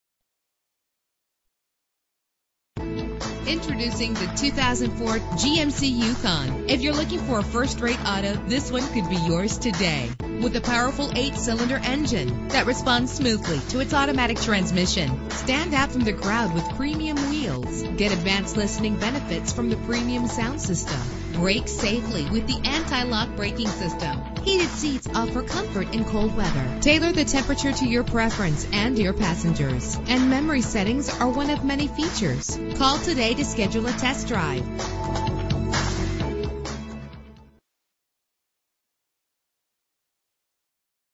2004 GMC Yukon - Elk Grove CA
Year: 2004
Make: GMC
Model: Yukon
Trim: Denali
Engine: 6.0 liter 8 cylinder 16 valve
Transmission: 4-Speed Automatic
Color: Silver Birch Metallic
Mileage: 92742
Address:
9776 W Stockton Blvd